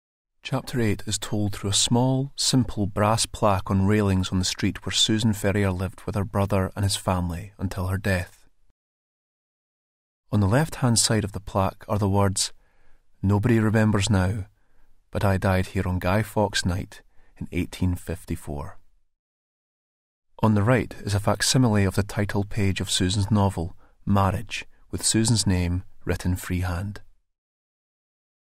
Message from the skies 2018 - Chapter 8 - Val McDermid - New Year's Resurrection
Message from the skies 2018

Chapter 8 of Val McDermid's New Year's Resurrection
38 Albany Street

Between 1818 and 1854, 38 Albany Street was occupied by members of the Ferrier family. In 1851, Susan Ferrier moved in to the house, after her health started to decline noticeably, and she lived there until her death on 5 November 1854.

Prior to her move, she lived on nearby Nelson Street, where she spent much of her time writing in darkened rooms due to the deterioration of her eyesight.

Albany Street has other literary connections, as it was also home to Mary Brunton, another successful 19th century novelist, and to James Ballantyne, Sir Walter Scott’s publisher.